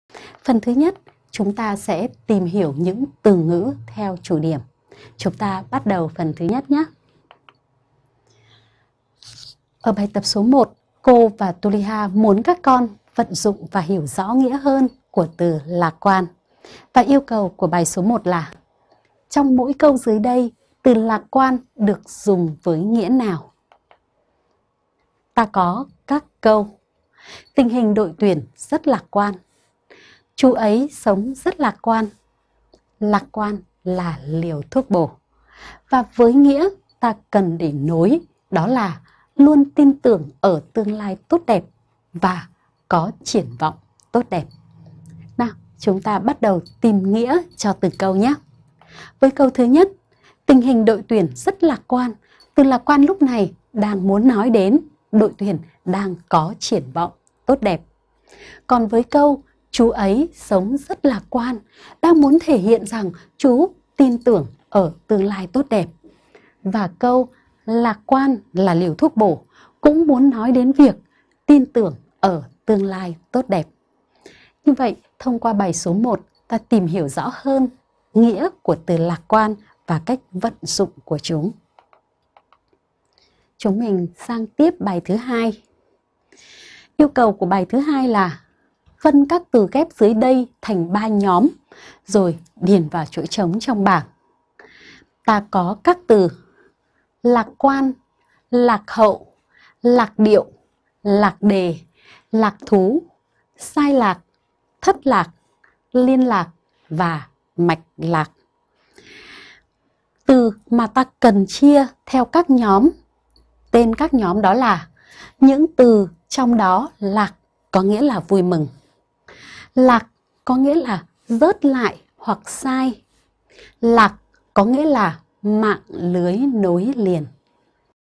Mở rộng vốn từ: Lạc quan - Yêu đời - Luyện từ và câu - Tiếng việt 4
Bài 1: Trong mỗi câu dưới đây, từ lạc quan được dùng với nghĩa nào?
          Câu                                                                         Nghĩa
Tình hình đội tuyển rất lạc quan.          Luôn tin tưởng ở tương lai tốt đẹp.
Chú ấy sống rất lạc quan.                      Có triển vọng tốt đẹp.
Lạc quan là liều thuốc bổ.
Xem đầy đủ bài giảng tại